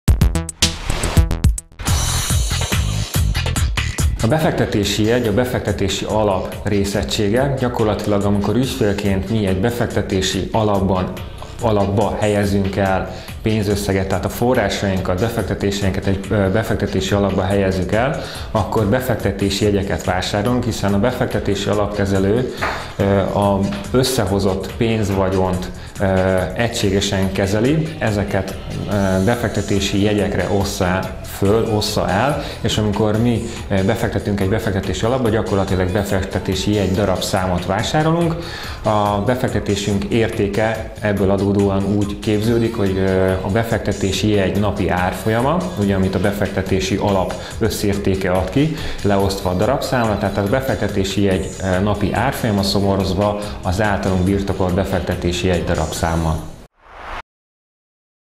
GazdaságTV.hu Brókervilág videószótár: Befektetési jegy
A befektetési jegy a befektetési alap részegységeként ismert. De lássuk, mit takar pontosan? A Quaestor nemzetközi üzletágvezetője, Illés László válaszol.